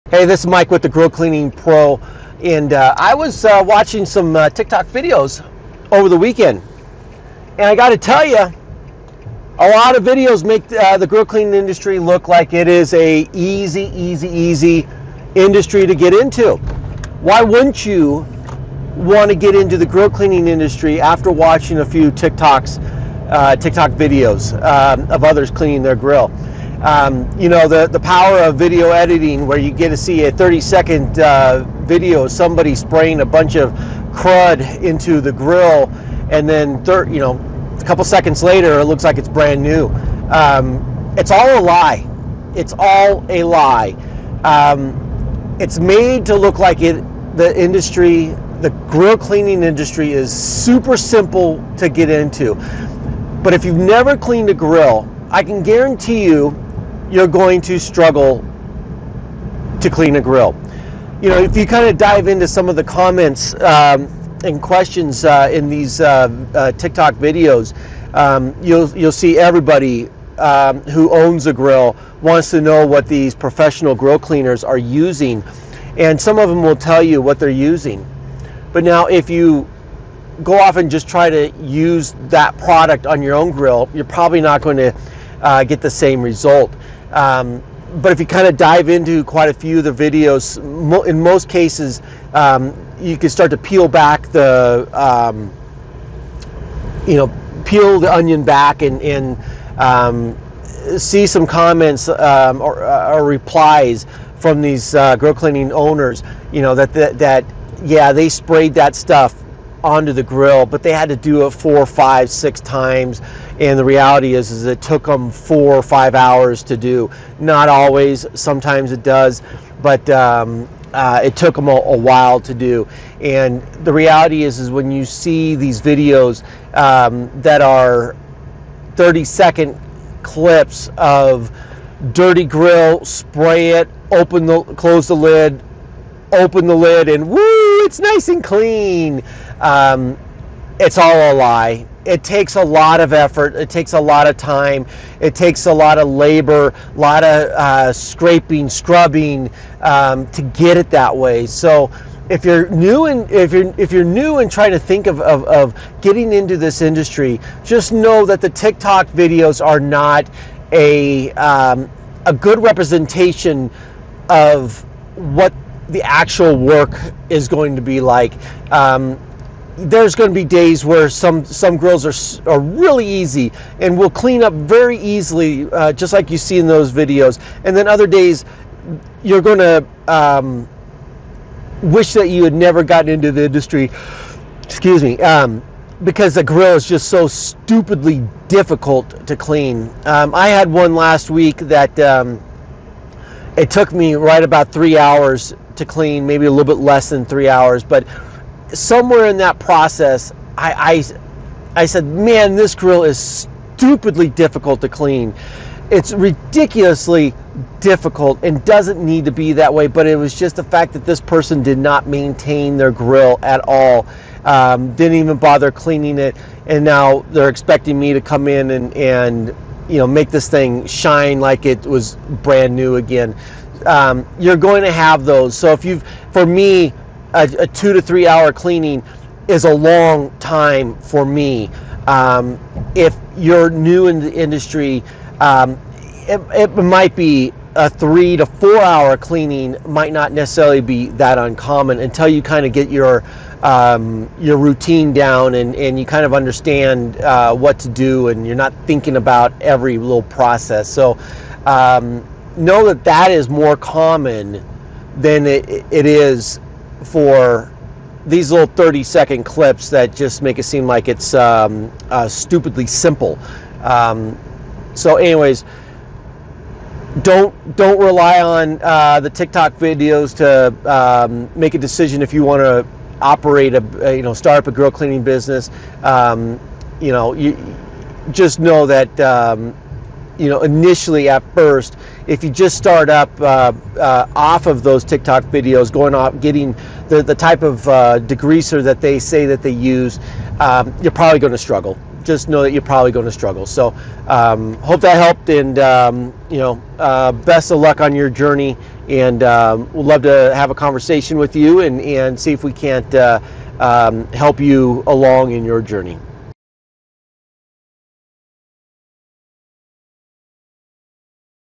Its all a lie
Do you watch the videos on TikTok and think to yourself the grill cleaning industry is easy work? Well, it's all a lie!